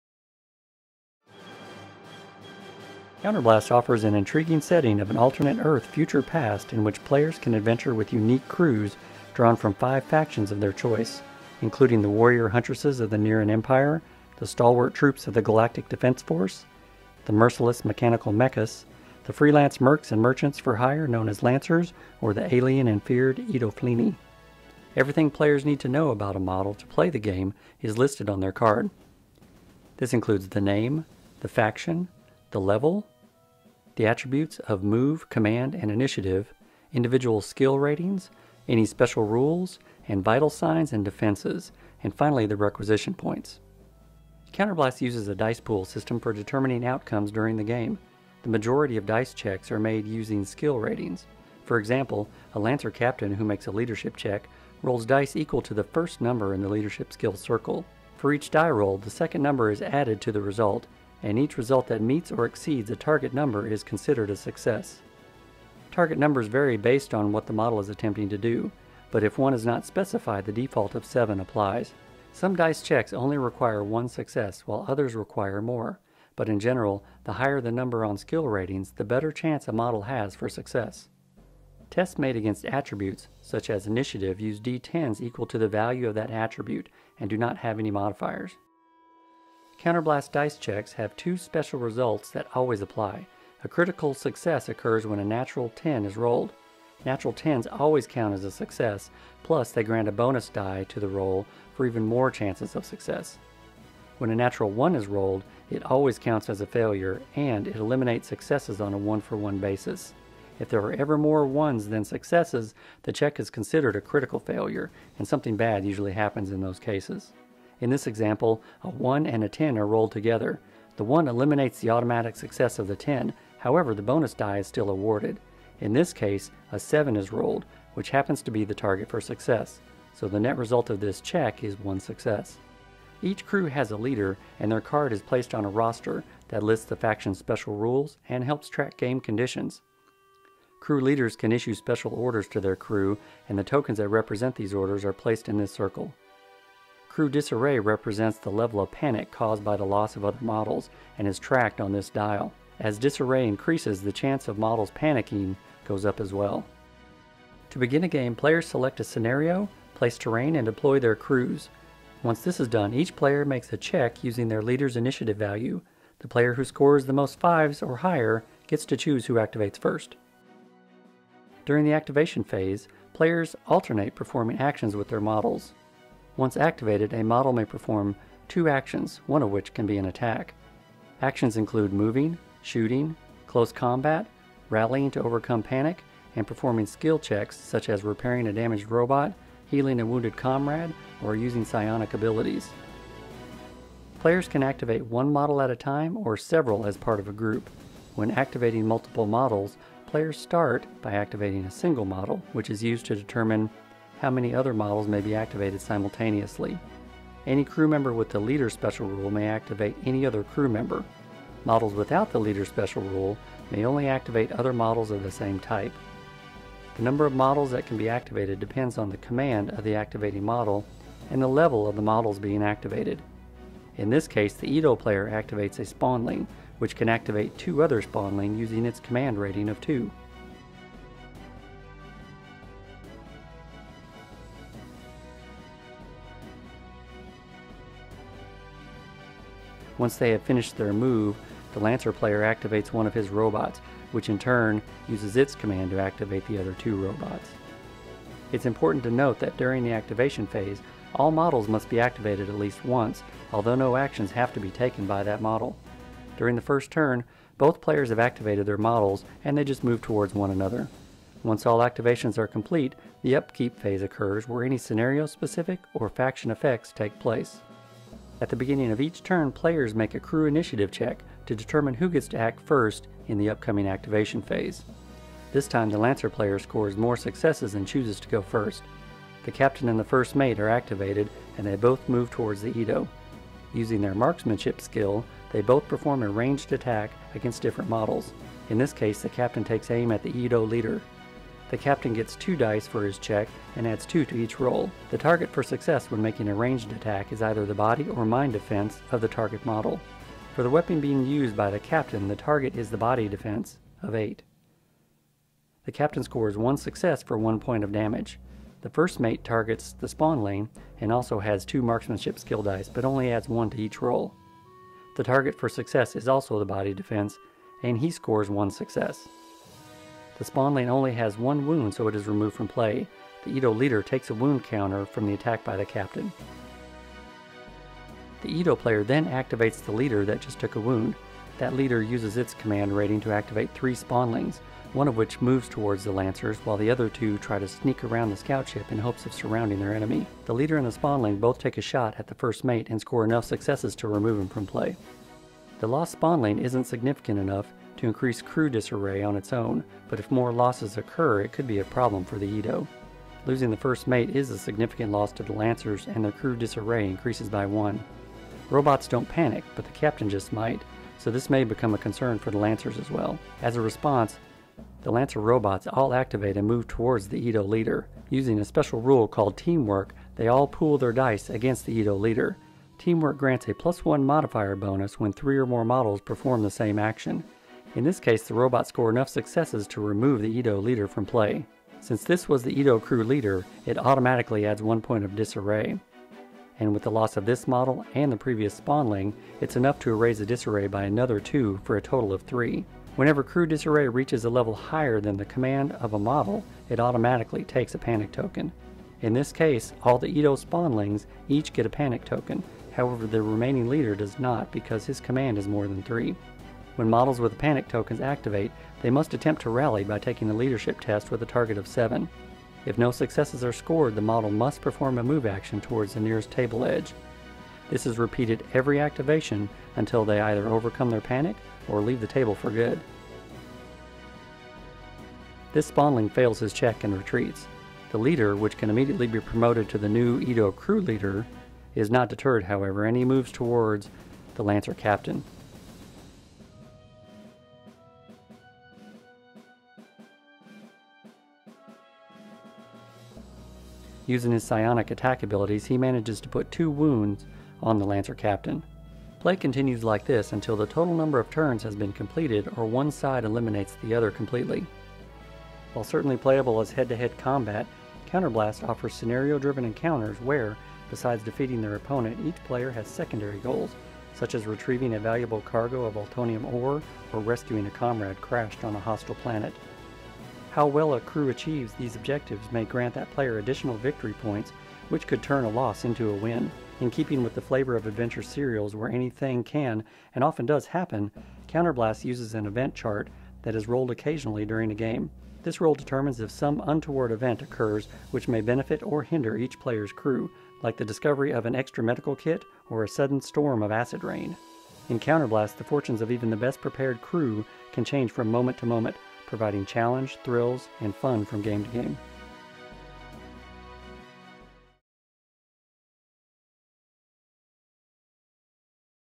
Counterblast Game Demo
Bombshell Miniatures is pleased to present the COUNTERBLAST Adventure Battle Game, a new pulp-inspired science fiction tabletop skirmish battle game for 28-32mm miniatures. COUNTERBLAST takes place in a setting of classic pulp sci-fi adventure with fast paced action and cinematic thrills, evocative of the serials that captivated the imagination of previous generations.